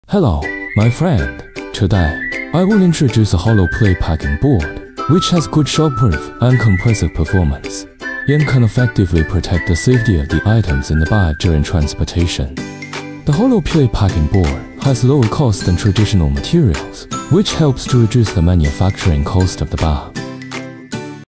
Coroplast sheet
Coroplast sheet can replace corrugated cardboard, wood, metal plate and other packaging materials, widely used in electronics, food, beverage, pesticides, precision instruments and other products packaging. In addition, hollow plates can also be made into turnover boxes, parts turnover boxes, food turnover boxes, etc.